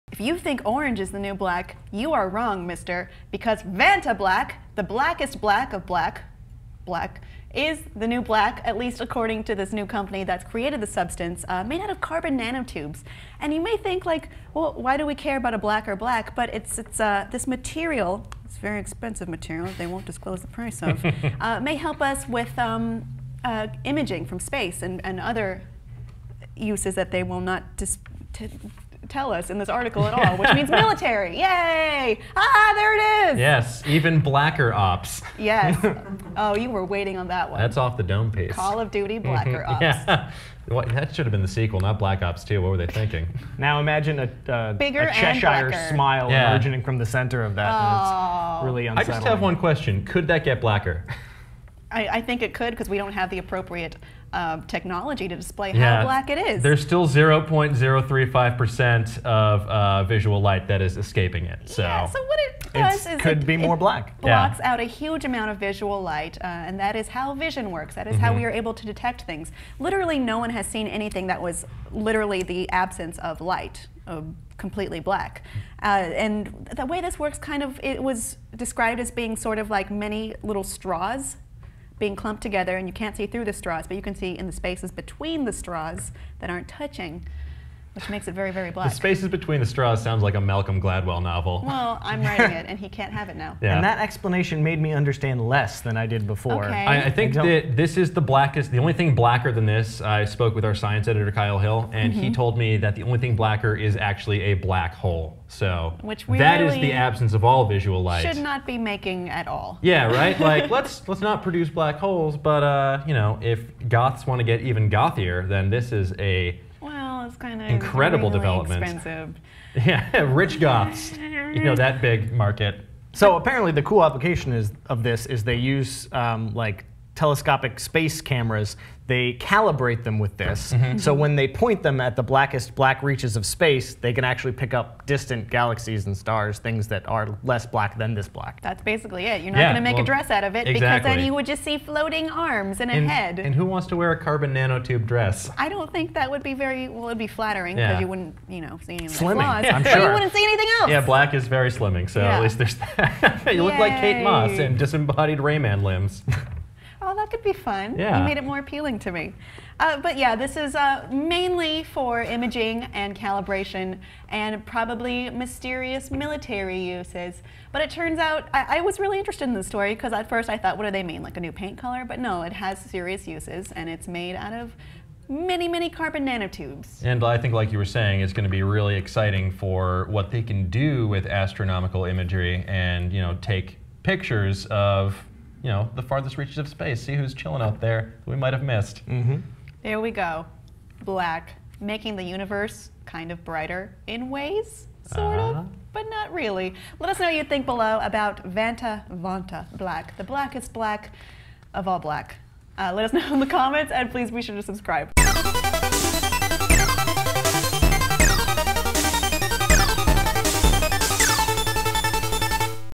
Vantablack - The Material So Dark You Can't See It
Vantablack is a material claiming to be the darkest shade of black possible-- it blocks out a massive amount of visible light, though obviously cannot be the complete absence of light. What is this extremely dark material, and why do scientists call it the "blackest black?" And besides sensitive imaging systems, what else could it be applied toward? Kim Horcher, Tim Frisch, and Dan Casey (Nerdist) discuss!

ON TWITTER: @NerdAlertTYT @kimscorcher @timmyfrisch @osteoferocious